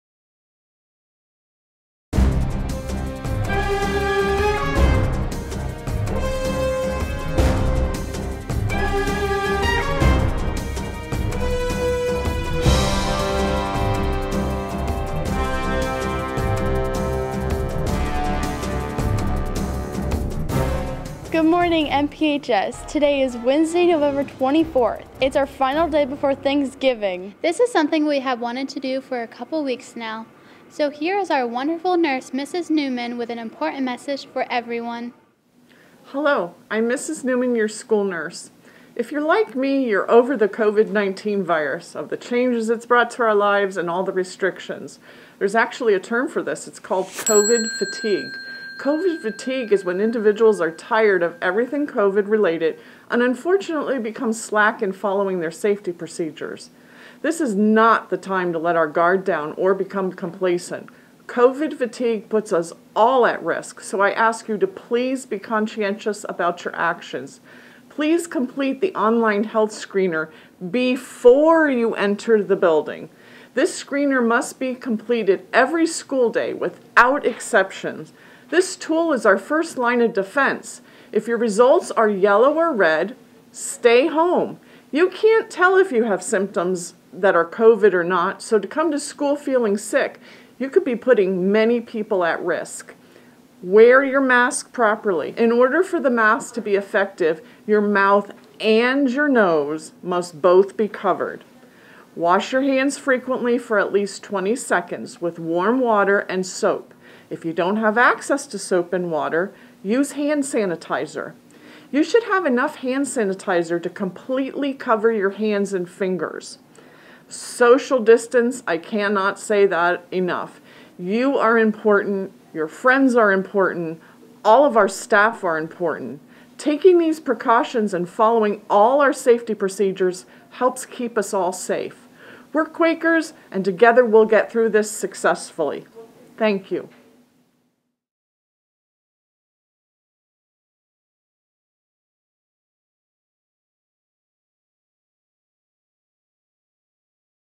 November 24, 2020
Morning Announcement Program for New Philadelphia High School in New Philadelphia, Ohio, USA
Featured is our school nurse, Mrs. Mary Newman, speaking about COVID Fatigue and items we must remember as we move forward through our school year.

Talent: Erika Kugler, Mallory Grace Johnson, John Reinard